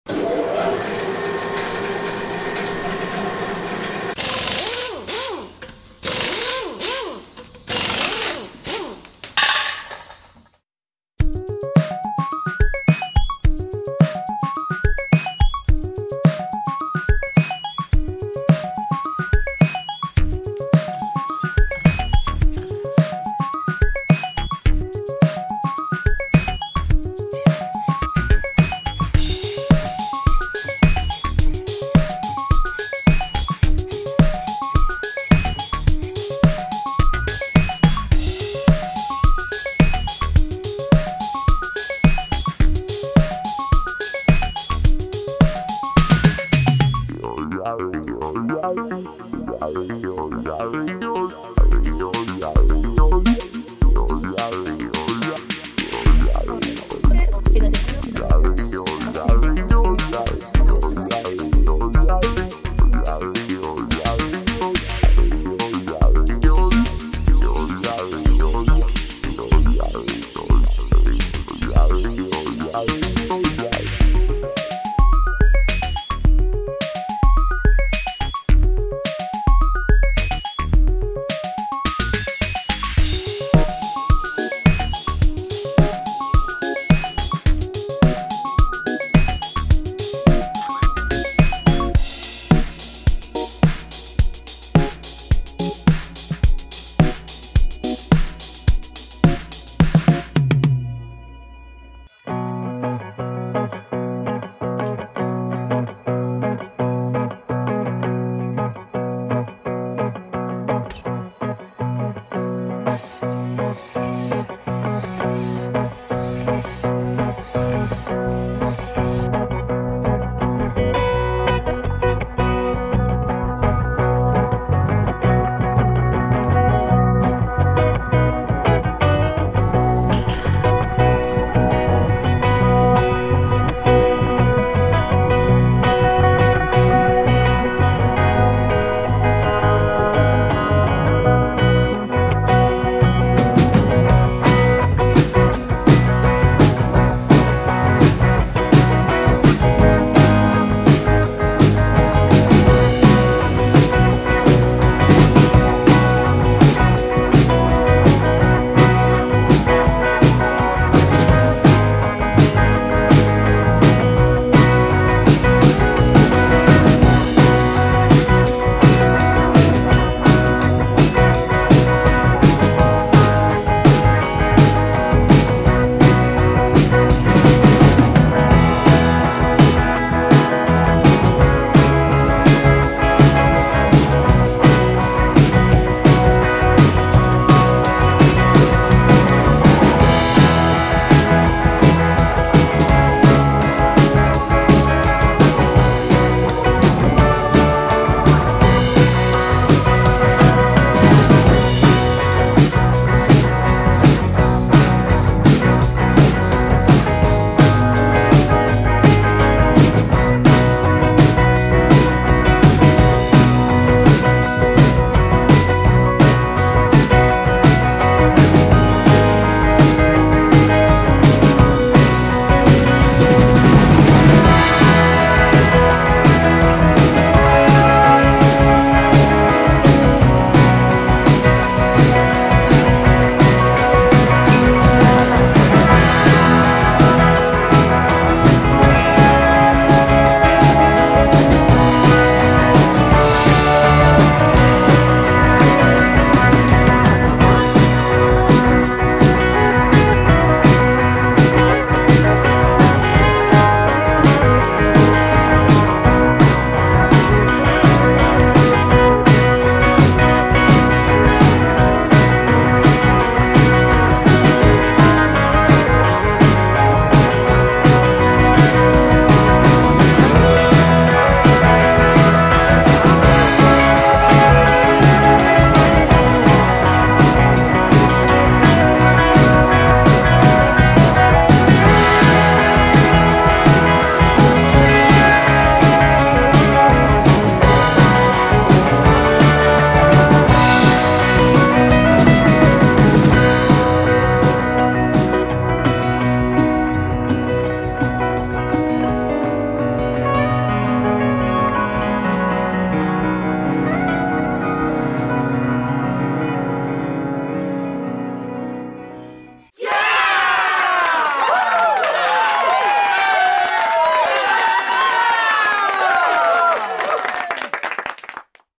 MIT 2.12 / 2.120 Two-Link Robot Arms in Motion (Spring 2021)
MIT 2.12 / 2.120 (Introduction to Robotics) video compilation from videos submitted by students showing their self-built 2-link robot arms drawing a circle and other interesting shapes. The joint angles are calculated based on the dry erase marker position generated at each time step using inverse kinematics. The two DC motors are controlled by an Arduino Uno / motor shield with a PID algorithm and encoder signal feedback.